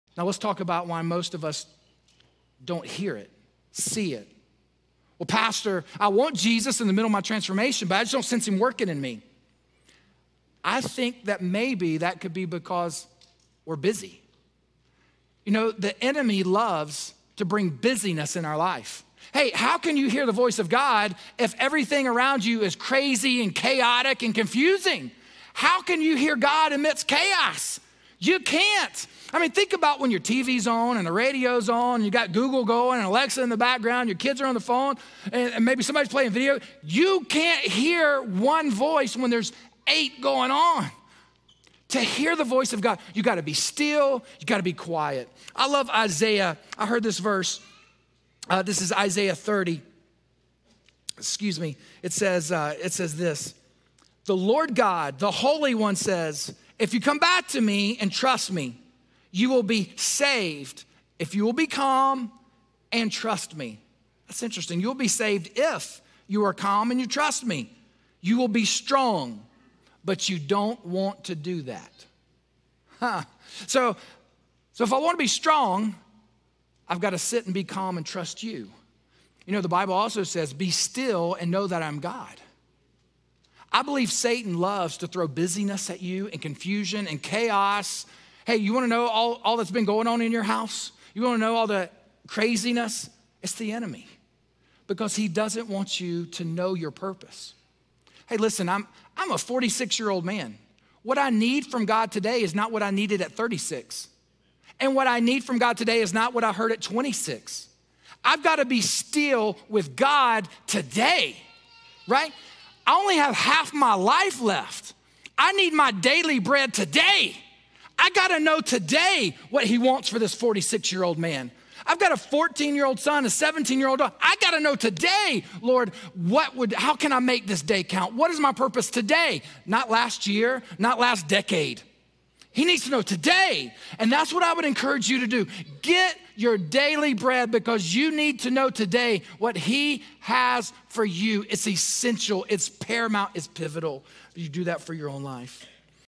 The Enemy Loves Busyness
Pastor Rick Julian from Sunday's sermon. Is Jesus working in you?